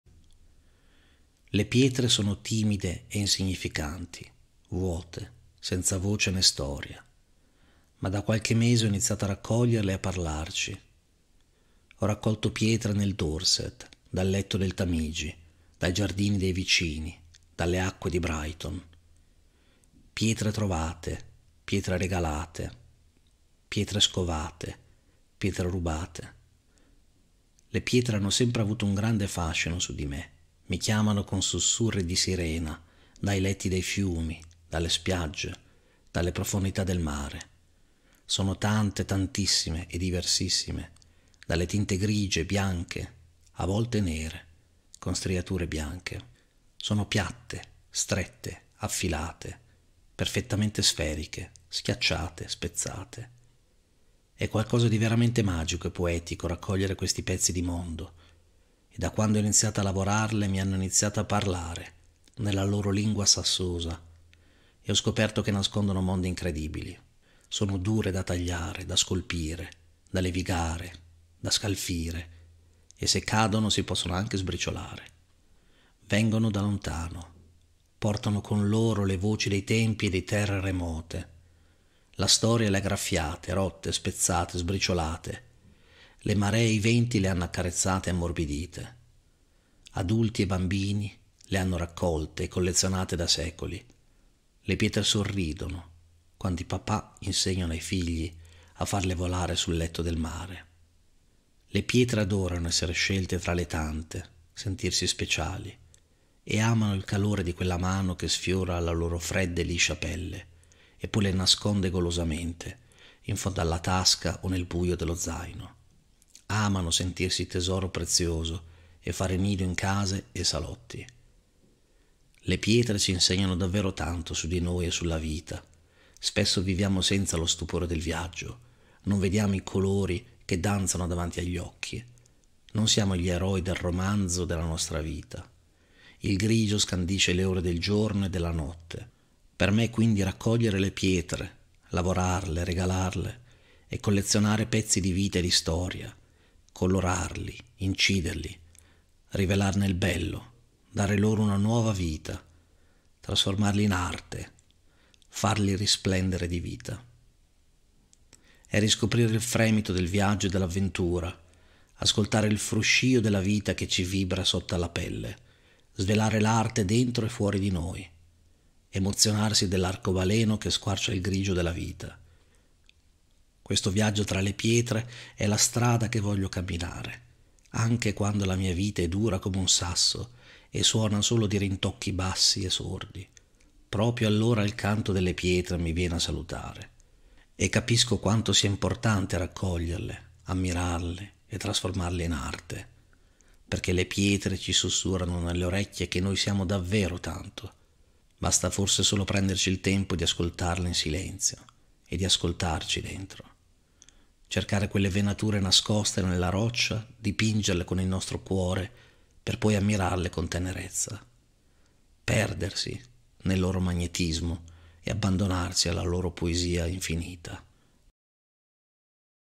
Le pietre preziose - una poesia di Riccardo Sai
✨ Le pietre preziose 🗿

Nella mia ultima poesia ho condiviso la mia passione per le pietre preziose e il loro potere poetico.

Ho raccolto pietre da ogni angolo del mondo e ho imparato a lavorarle, scoprendo mondi incredibili nascosti in esse.

Le pietre ci insegnano tanto sulla vita e su di noi stessi, e io ho deciso di trasformarle in arte, rivelando il loro splendore e ascoltando il loro eterno canto.

Seguite il mio viaggio tra le pietre e scoprite l'importanza di fermarsi a ascoltare e ammirare la poesia infinita che esse ci sussurrano.

Cosa credi che le pietre possano insegnarci sulla vita e sulla nostra relazione con il mondo che ci circonda?

Come pensi che la raccolta e la lavorazione delle pietre possano essere un'illustrazione della trasformazione interiore e della capacità di trovare bellezza nell'imperfezione?

Come le pietre possono ispirare il sentimento di avventura e meraviglia nella vita quotidiana?